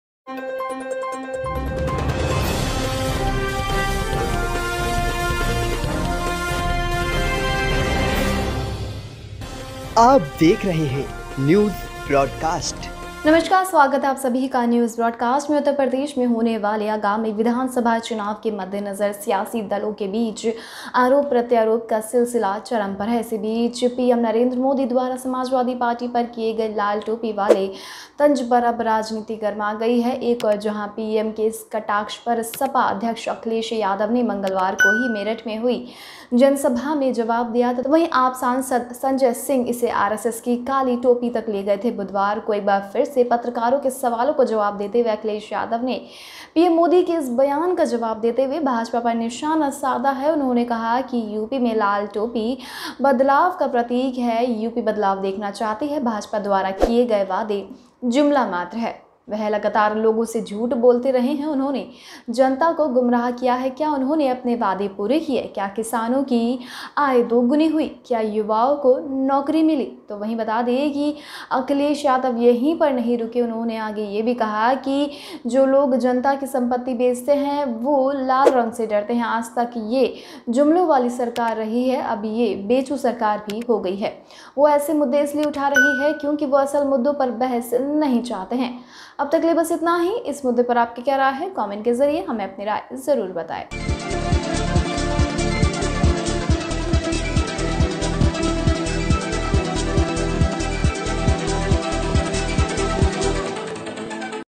अखिलेश ने लाल टोपी को लेकर PM मोदी पर किया पलटवार | Akhilesh Yadav | Narendra Modi
उत्तर प्रदेश में होने वाले आगामी विधानसभा चुनाव के मद्देनजर सियासी दलों के बीच आरोप-प्रत्यारोप का सिलसिला चरम पर है. इसी बीच प्रधानमंत्री नरेंद्र मोदी द्वारा समाजवादी पार्टी पर किए गए 'लाल टोपी' वाले तंज पर अब राजनीति गरमा गई है। एक ओर जहां पीएम के इस कटाक्ष पर सपा अध्यक्ष अखिलेश यादव ने मंगलवार को ही मेरठ में हुई जनसभा में जवाब दिया था, वहीं आप सांसद संजय सिंह इसे RSS की 'काली टोपी' तक ले गए थे। बुधवार को एक बार फिर पत्रकारों के सवालों का जवाब देते हुए अखिलेश यादव ने पीएम मोदी के इस बयान का जवाब देते हुए भाजपा पर निशाना साधा है।